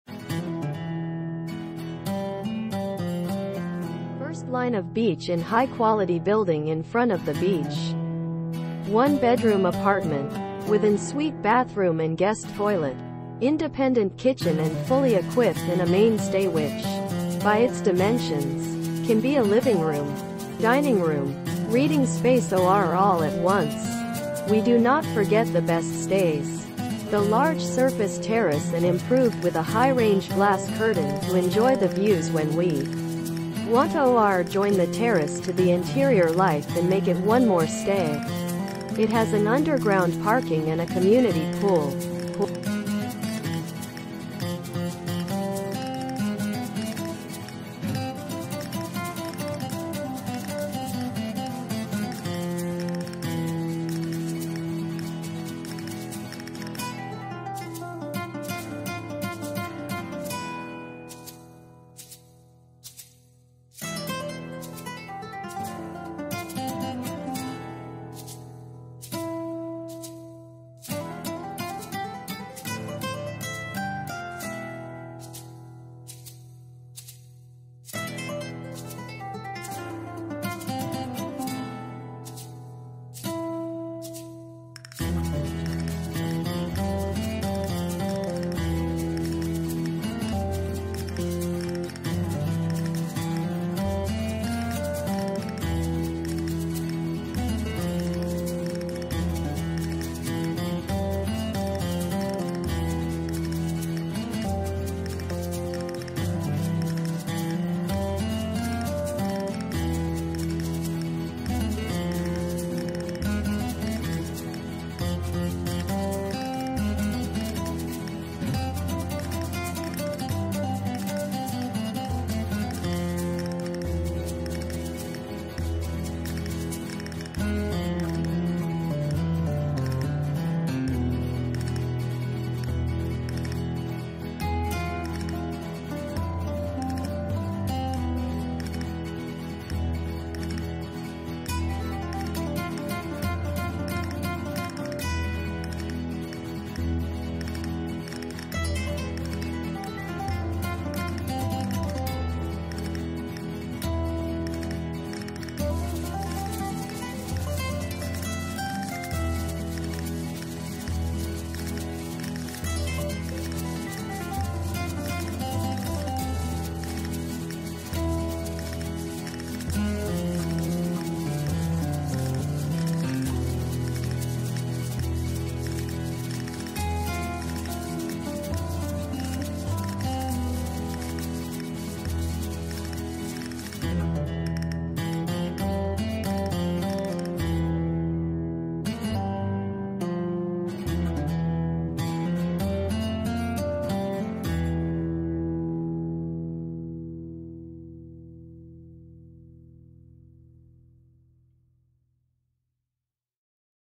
Middle Floor Apartment in Fuengirola, Costa Del Sol FOR SALE €440000
Middle Floor Apartment in Fuengirola, FOR SALE €440000 Property ID: CDS19331
 FIRST LINE OF BEACH IN HIGH QUALITY BUILDING IN FRONT OF THE BEACH.
ONE BEDROOM APARTMENT, WITH AN SUITE BATHROOM AND GUEST TOILET, INDEPENDENT KITCHEN AND FULLY EQUIPPED AND A MAIN STAY WHICH, BY ITS DIMENSIONS, CAN BE A LIVING ROOM, DINING ROOM, READING SPACE OR ALL AT ONCE, WE DO NOT FORGET THE BEST STAYS, THE LARGE SURFACE TERRACE AND IMPROVED WITH A HIGH-RANGE GLASS CURTAIN TO ENJOY THE VIEWS WHEN WE WANT TO OR JOIN THE TERRACE TO THE INTERIOR LIFE AND MAKE IT ONE MORE STAY. IT HAS AN UNDERGROUND PARKING AND A COMMUNITY POOL.
Covered Terrace,Lift,Fitted Wardrobes,Near Transport,Private Terrace,WiFiBeachfront,Town,Commercial Area,Close To Shops,Close To Schools,Urbanisation